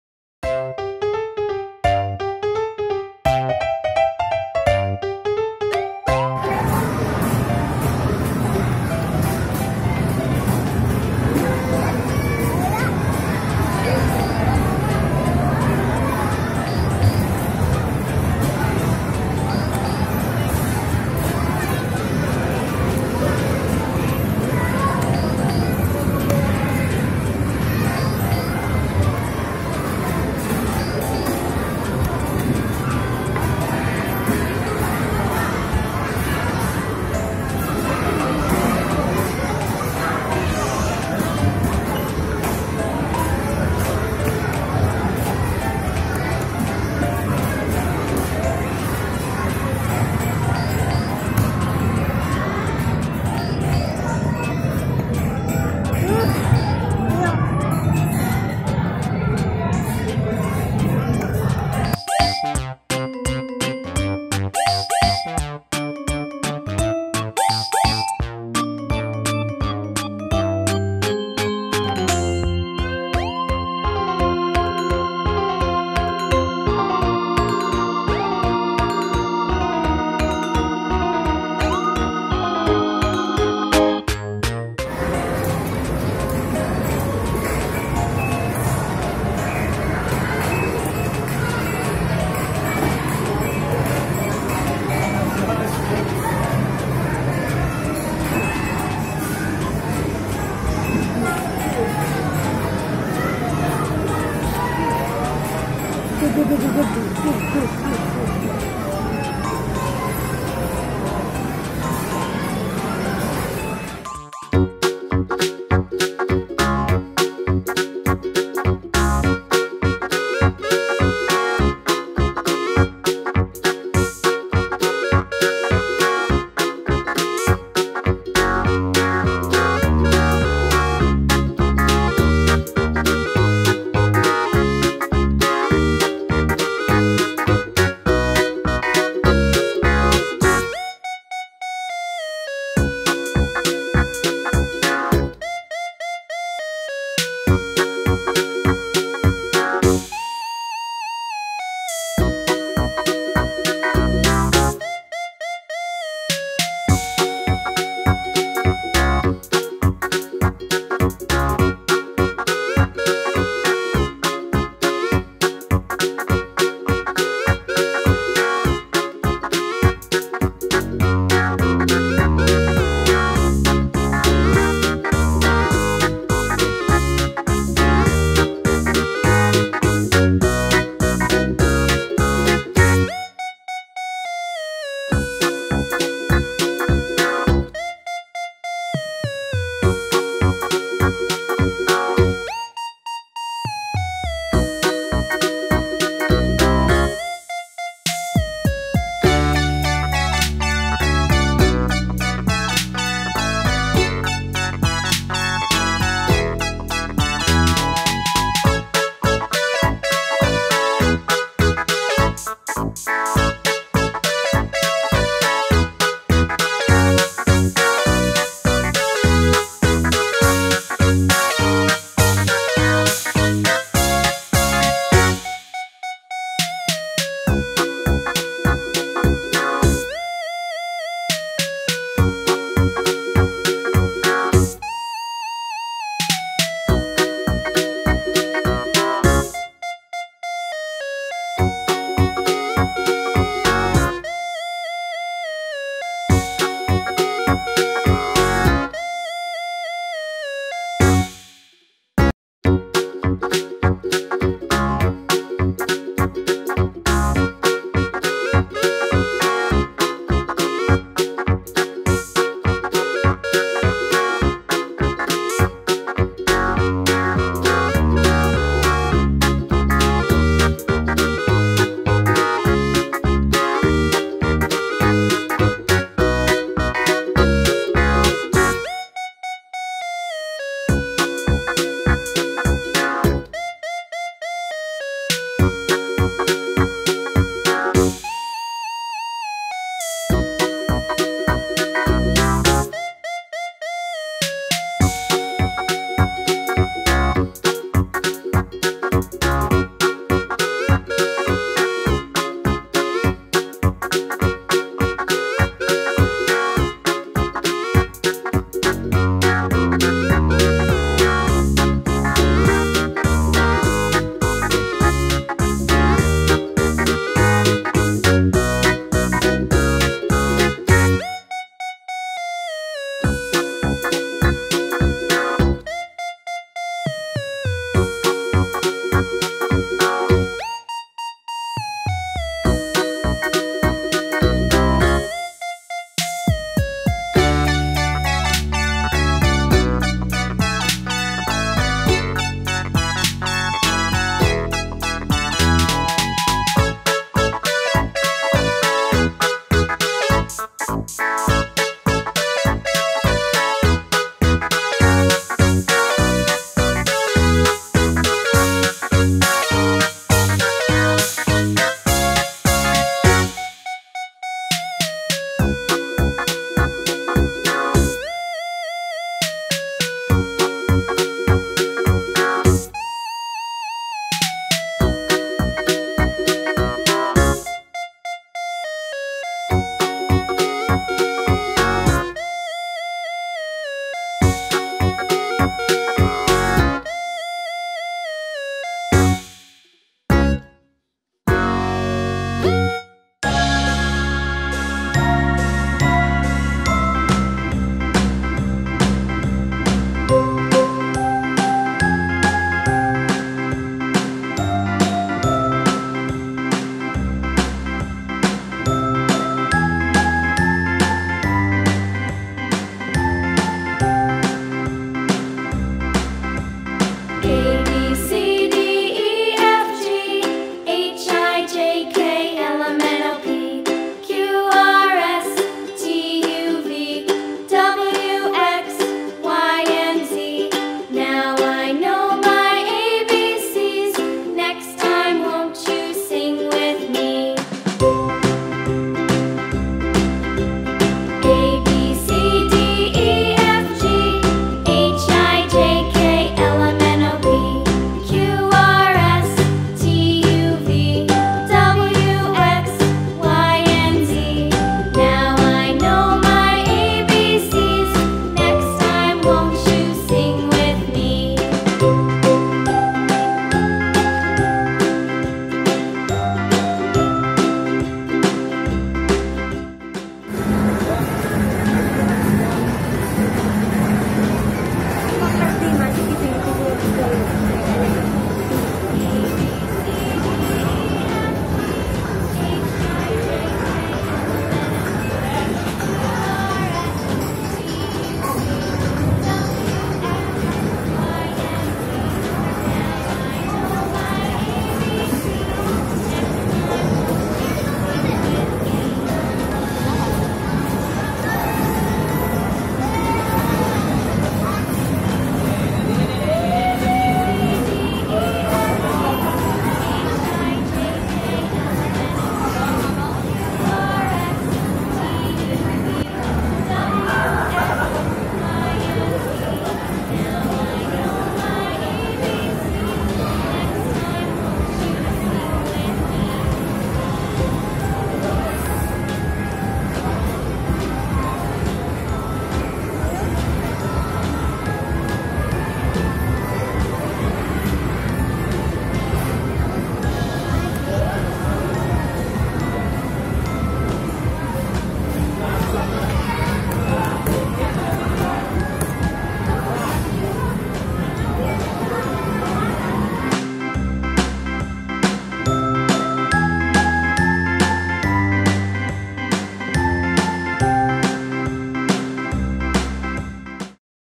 Habis Lihat Barongsai Lanjut Main ke Timezone Margo City Depok
Vano habis nonton aktraksi barongsai, lanjut main ke Timezone Margo City, Depok